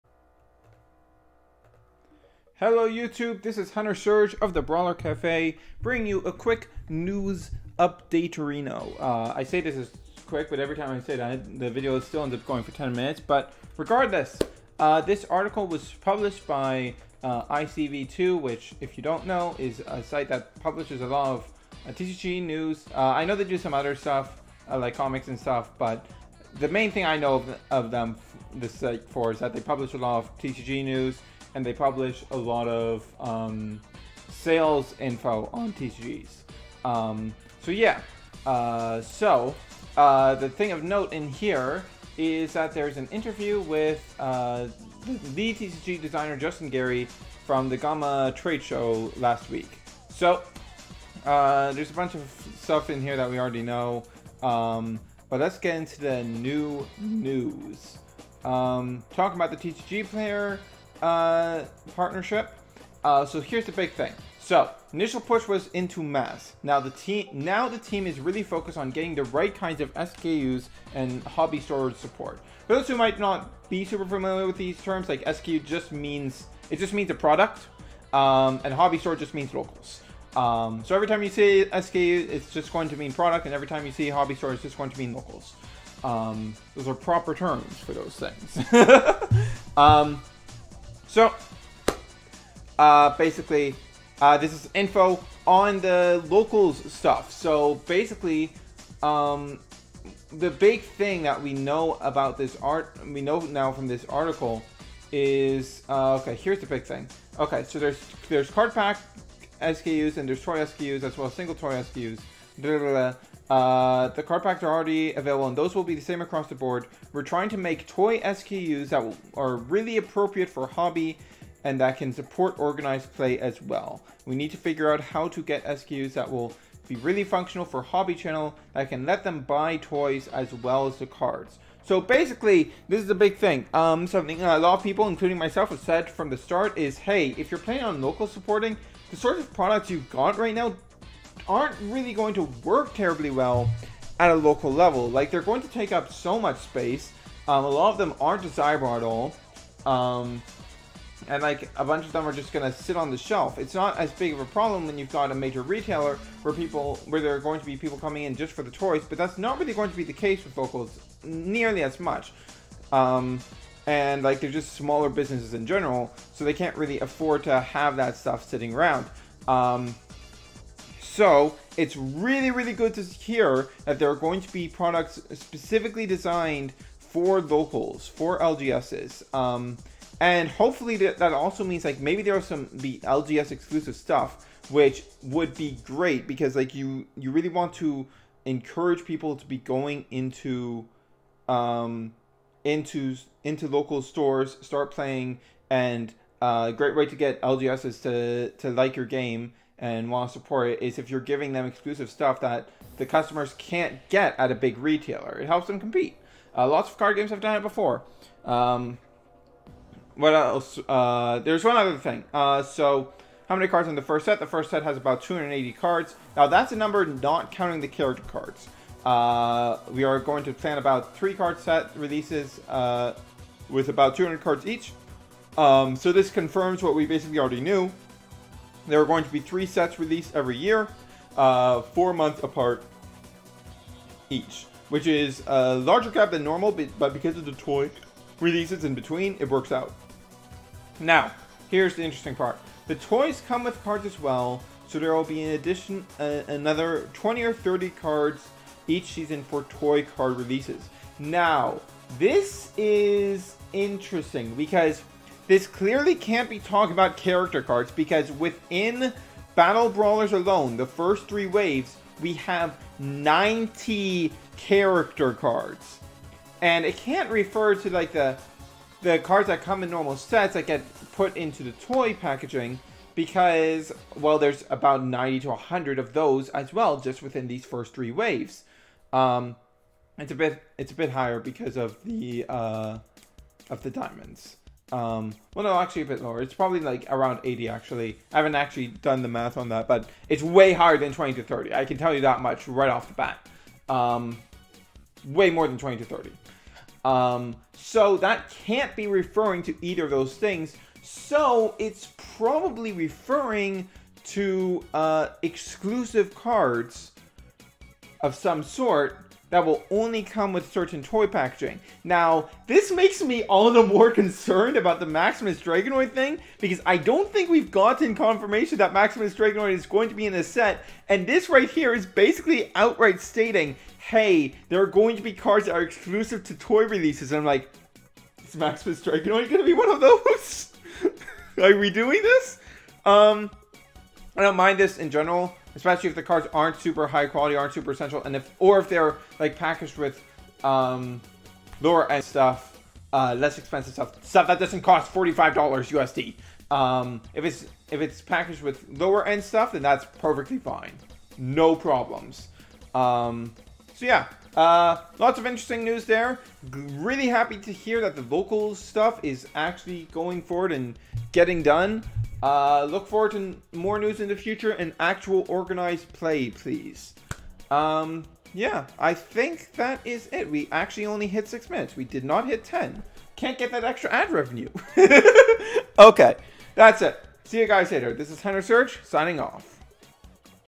New LGS Information! Toy-Exclusive Cards? | Bakugan Battle Planet
Just quick news update going over the LGS info confirmed in the ICv2 article that dropped yesterday!